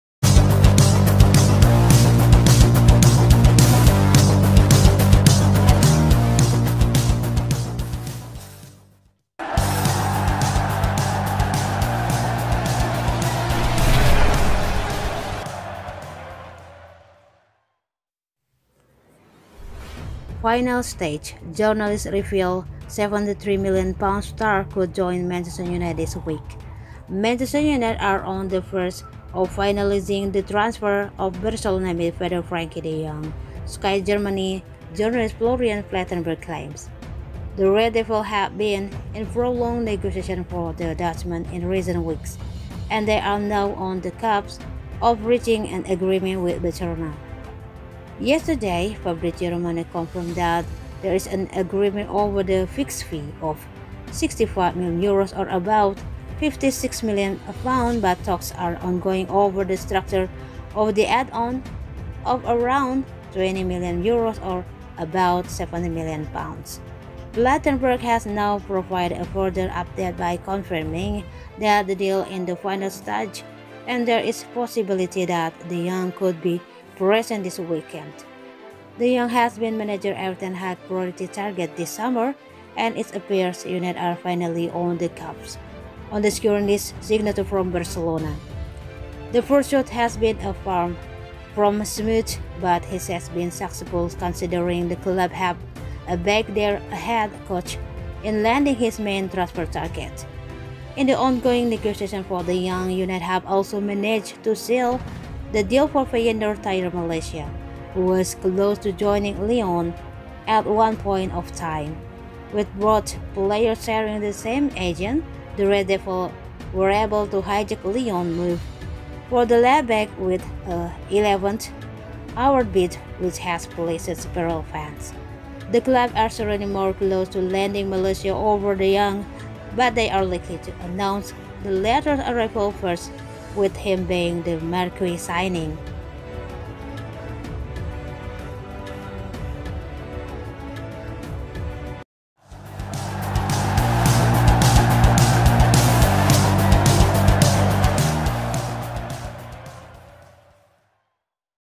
'Final stages': Journalist reveals £73m star finally join Man Utd this week
'Final stages': Journalist reveals £73m star could join Man Utd this week

Manchester United are on the verge of finalising the transfer of Barcelona midfielder Frenkie de Jong, Sky Germany journalist Florian Plettenberg claims.

The Red Devils have been in prolonged negotiations for the Dutchman in recent weeks, and they are now on the cusp of reaching an agreement with Barcelona.

Yesterday, Fabrizio Romano confirmed that there is an agreement over the fixed fee of €65 million (£56m), but talks are ongoing over the structure of the add-ons of around €20m (£17m).

Plettenberg has now provided a further update by confirming that ‘the deal in the final stages’ and there is a possibility that De Jong could be presented this weekend.

De Jong has been manager Erik ten Hag’s priority target this summer, and it appears United are finally on the cusp of securing his signature from Barcelona.

The pursuit has been far from smooth, but it has been successful, considering the club have backed their head coach in landing his main transfer target.

In the ongoing negotiations for De Jong, United have also managed to seal the deal for Feyenoord’s Tyrell Malacia, who was close to joining Lyon at one point of time.

With both players sharing the same agent, the Red Devils were able to hijack Lyon’s move for the left-back with a 11th hour bid, which has pleased several fans.

The club are currently more close to landing Malacia over De Jong, but they are likely to announce the latter’s arrival first with him being the marquee signing.